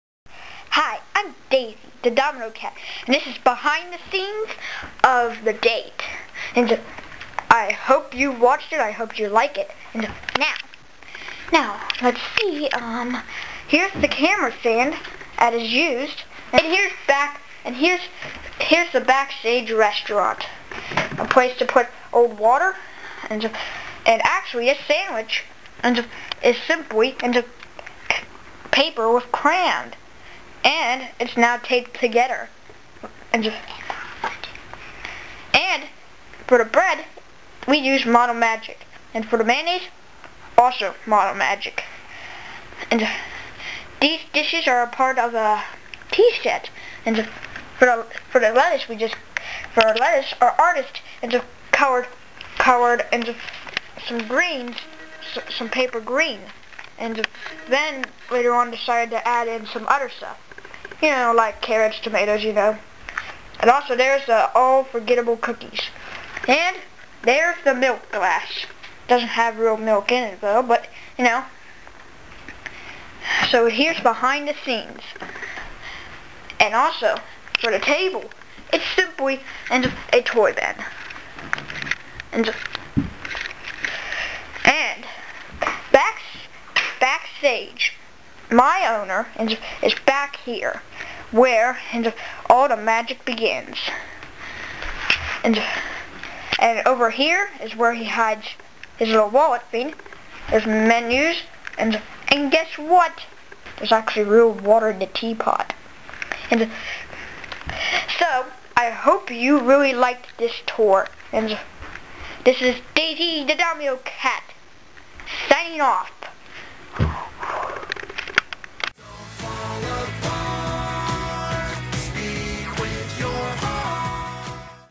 Webkinz go on a Date Back Stage Tour
This is anther old video my brother Dukecool15 did.